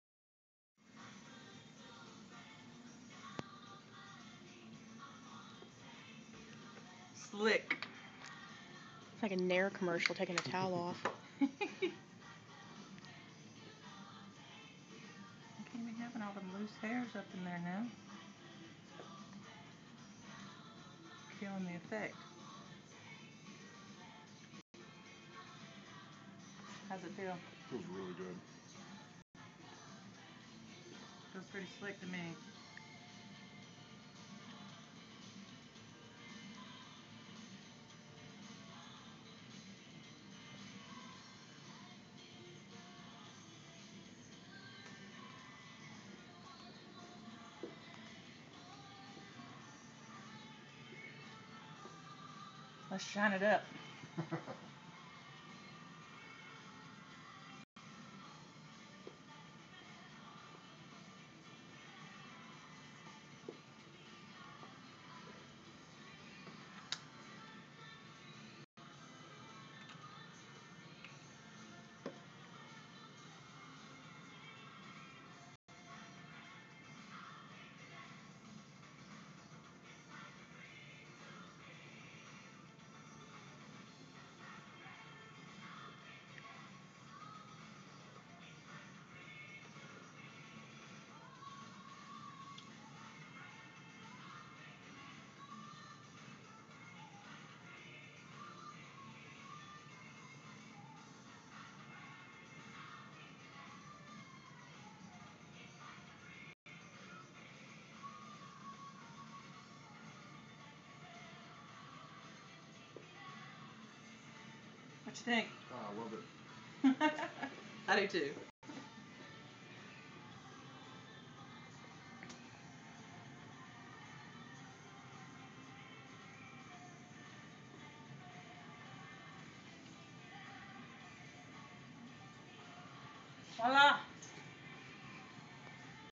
The Edge Barbershop straight razor head shave part3
Straight razor head shave. AJ getting his head shaved at The Edge Barbershop.